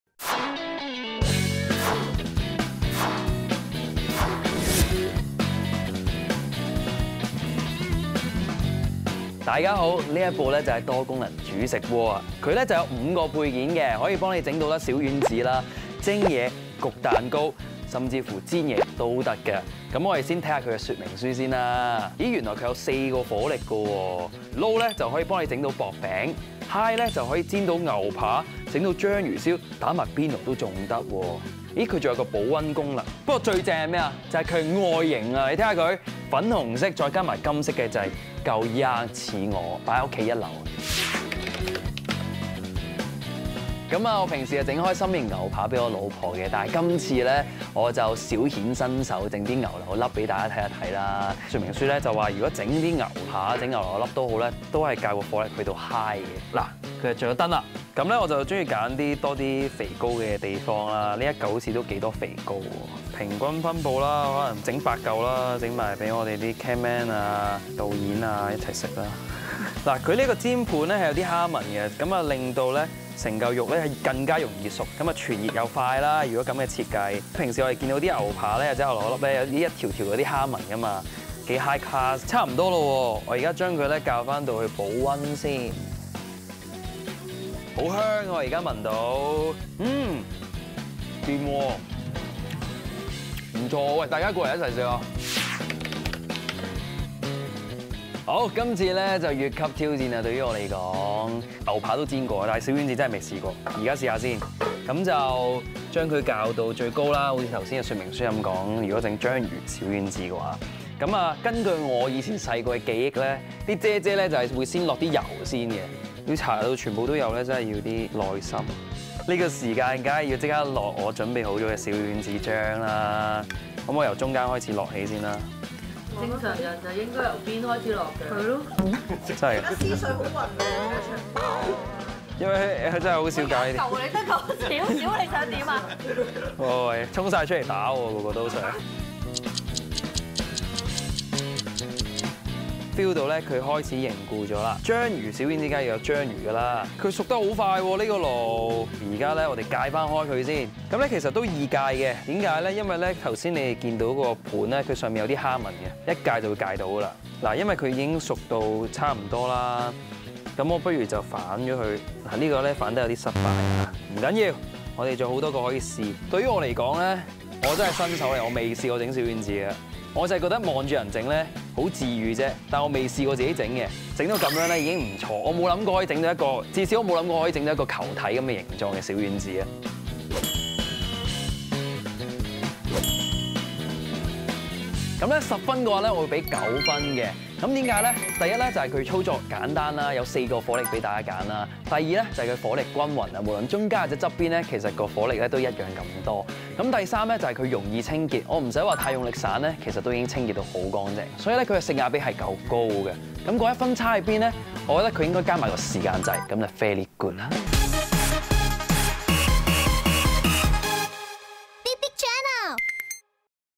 安樂蝸｜一個爐有咁多功能！｜實測｜多功能｜煮食爐｜林正峰
【安樂蝸】一個爐有咁多功能！

而家香港屋又細，地方又小，無可能焗蛋糕又買焗爐，蒸魚又買個蒸爐！妄購KOL阿楓介紹嘅多功能煮食爐就可能係你好幫手，不如睇吓佢用後有咩評價！